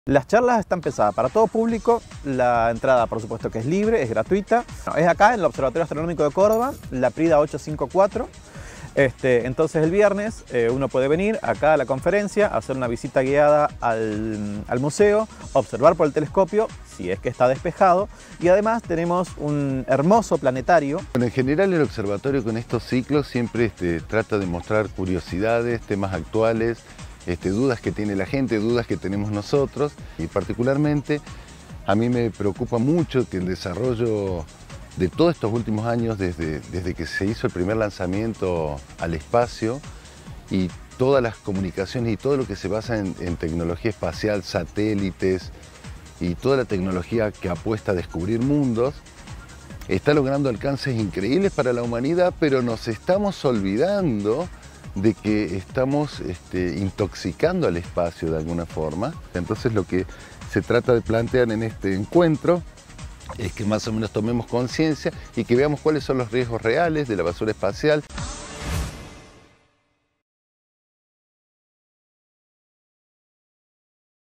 CHARLAS ABIERTAS EN EL OAC
En ese sentido el Observatorio Astronómico de Córdoba (OAC) viene desarrollando desde hace mas de 5 años este ciclo de conferencias científicas destinadas a todo público en las cuales se tratan entre otros diversos temas vinculados a las investigaciones que se desarrollan es esta institución científica con más de 147 años de experiencia.

Además, desde las 20 hasta las 23hs, se realizarán recorridos guiados por el museo, observaciones por un telescopio, si el clima lo permite, y funciones de Planetario.